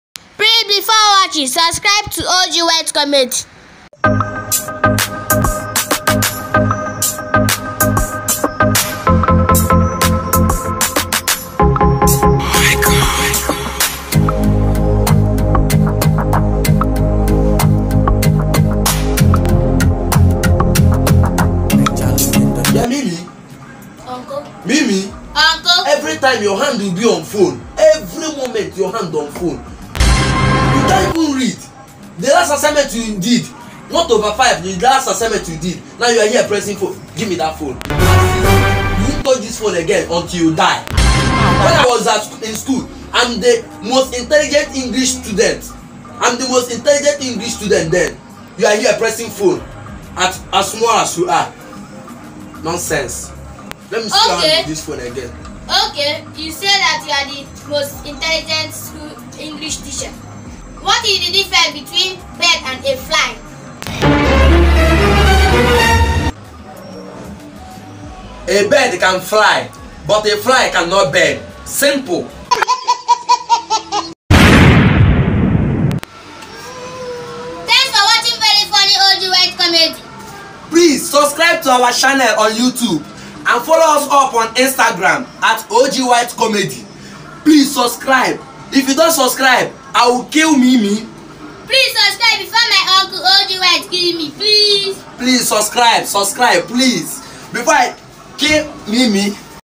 OG_wyt comedy (og 18) DIFFERENCES
OG claims to be the most intelligent English student but answered a wrong answer 🤣🤣🤣